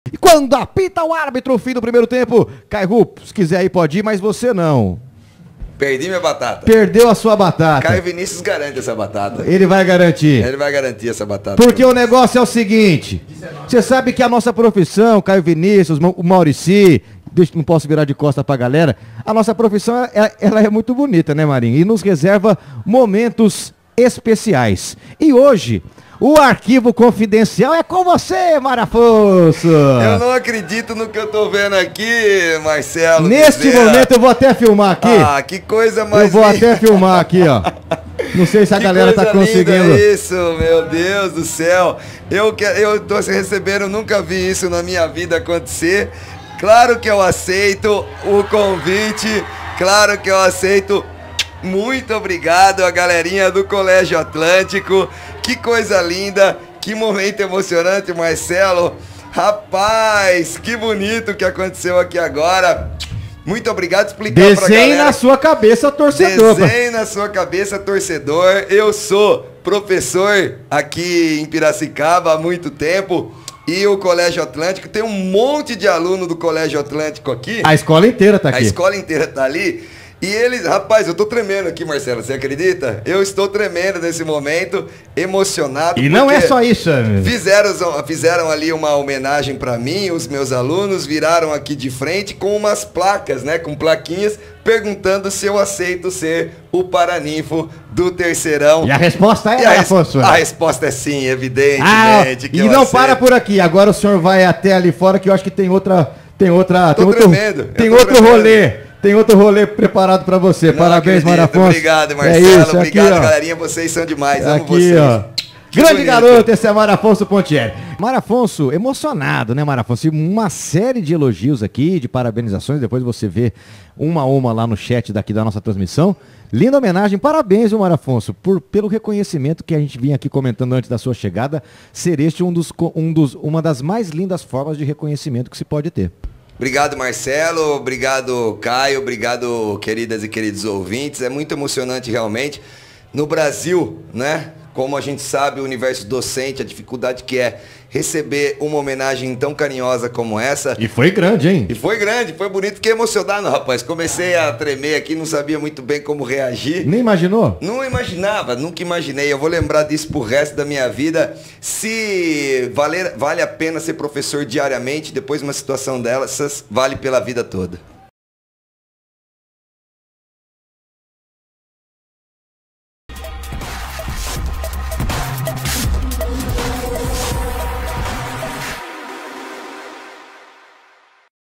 CONVITE - Alunos do colégio Atlântico convidam o Professor Mário Afonso para ser o Paraninfo
Um Convite Surpreendente: Alunos do colégio Atlântico, durante o intervalo do jogo entre XV de Piracicaba e Oeste, convidam o Professor Mário Afonso, repórter e comentarista da Rádio Piramundo, para ser o Paraninfo da turma, no estádio Barão de Serra Negra, Piracicaba-SP